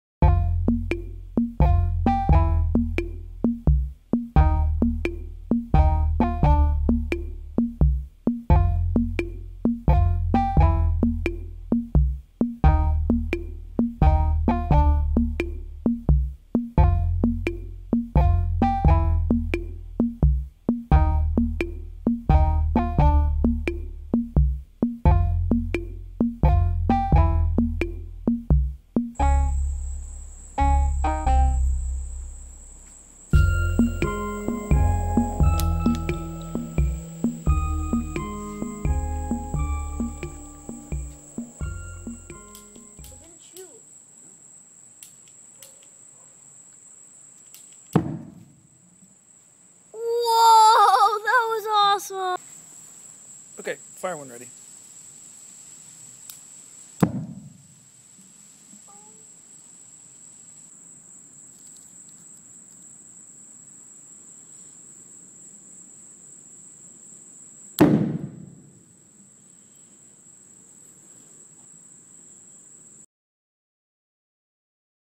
Carbide Mortar
This is a short movie of a calcium carbide mortar/cannon that I made for the 4th of July. It can be pretty loud...which, I guess, is the point.